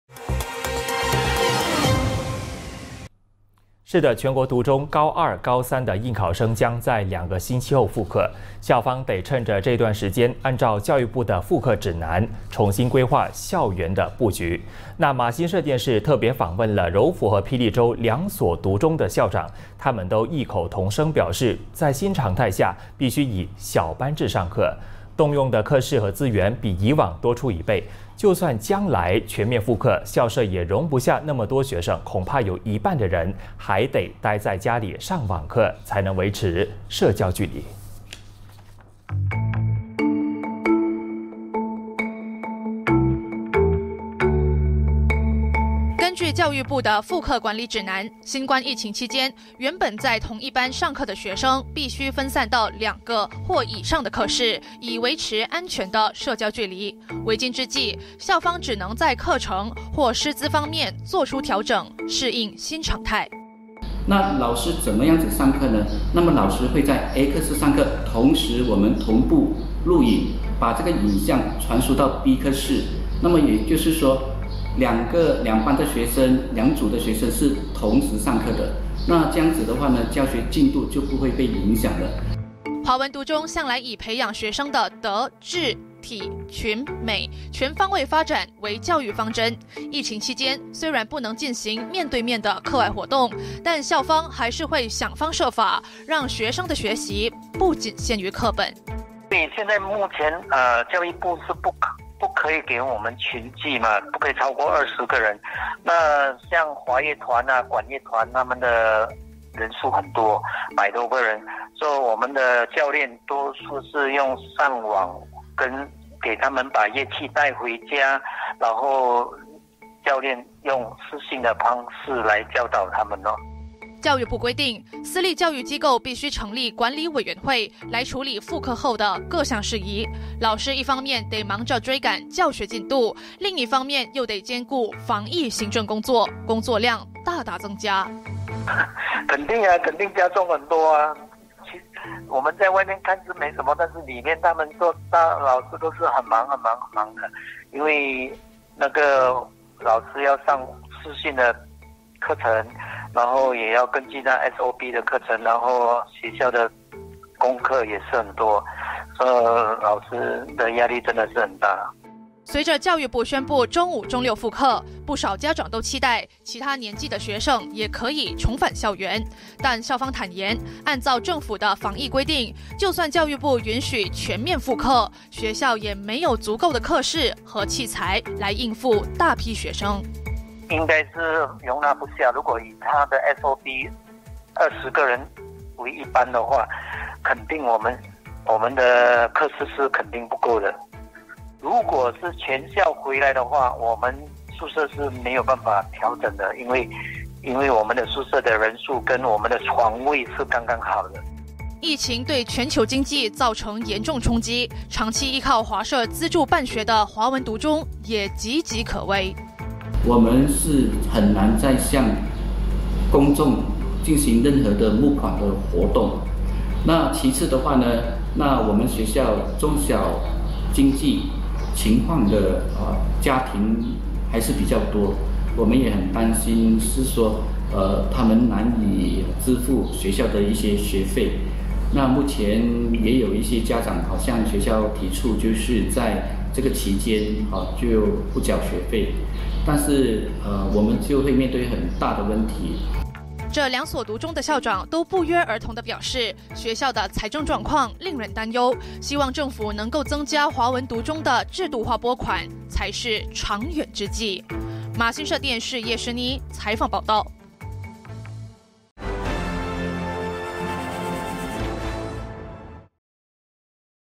20200611 规划校园布局准备复课 独中小班制耗人力设备
全国独中高二和高三的应考生将在两个星期后复课，校方得趁着这段时间，按照教育部的复课指南，重新规划校园布局。马新社电视特别访问了柔佛和霹雳州两所华文独中的校长，他们都异口同声表示，新常态下必须以小班制上课，动用的课室和资源比以往多出一倍。就算将来全面复课，校舍也容不下那么多学生，恐怕有一半的人得待在家里上网课，才能维持社交距离。

记者：叶诗妮